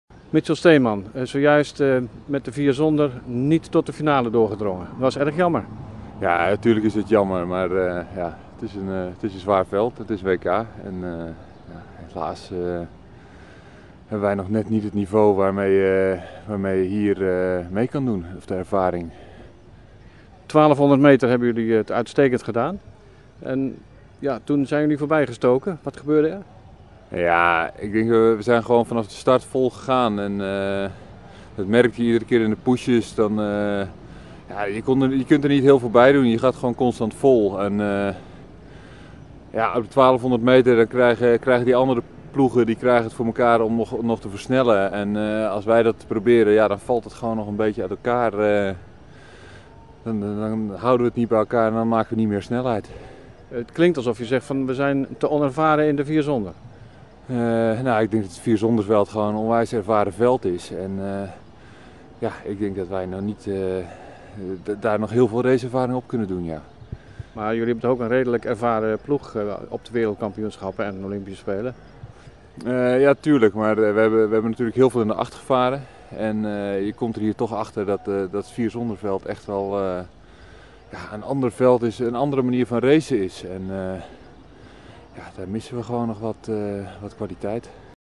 RoeiTv: M4- moet genoegen nemen met B-finale
De vier-zonder-stuurman met Olaf van Andel, Robert Lücken, Mechiel Versluis en Mitchel Steenman ging de strijd aan met de heersende wereldtop. Na een sterke start werd het een vierde plaats in de herkansing en een verwijzing naar de B-finale.